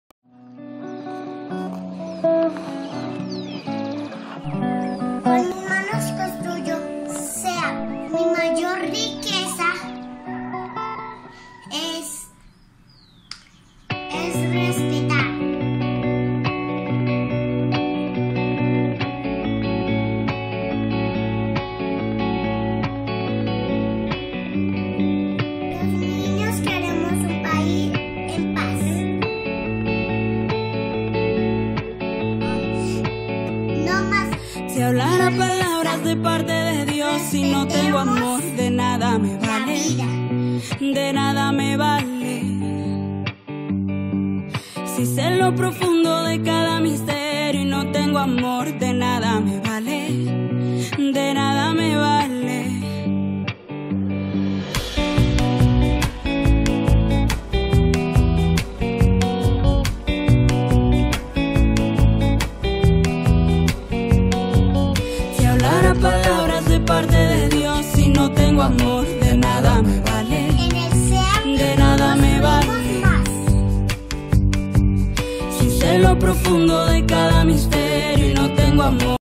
Mis Manos Construyen
El Sistema Educativo de la arquidiócesis de Bogotá (SEAB) invita a la “velatón”  las 7:00 p.m., de manera virtual para expresar tu sentir por los acontecimientos que vivimos actualmente y para pedir por la paz y la reconciliación de Colombia.